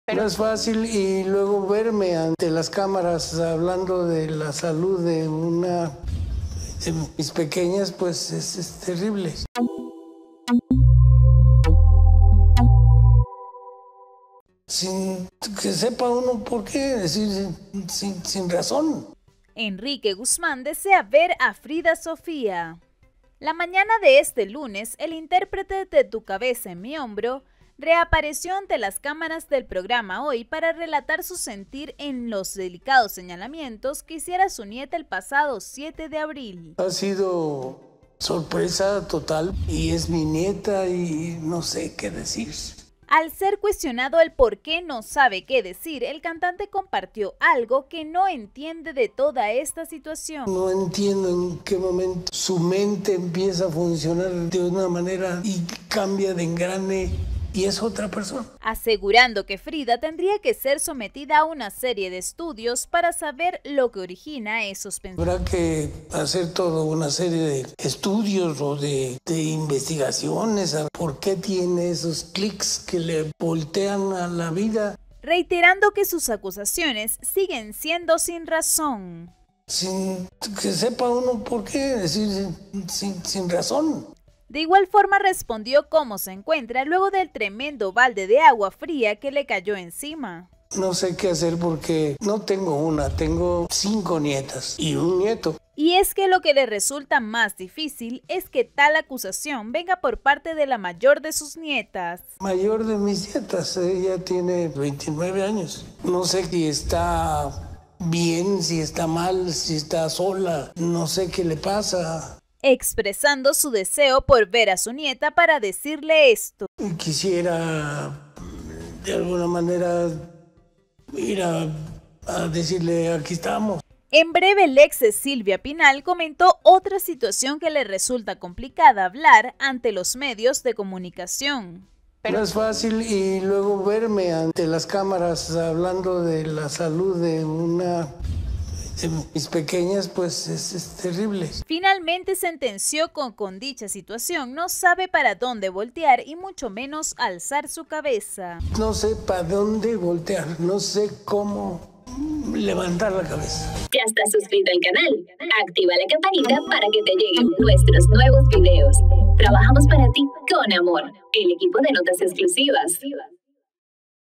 Enrique Guzmán desea ver a Frida Sofía
MFL NETWORKS INC.